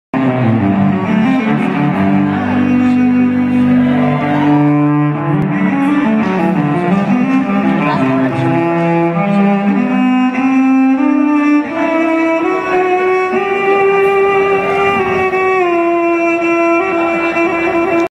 Third World reggae band cellist performance
Third World member Stephen Coore plays a moving solo at its Concert at York College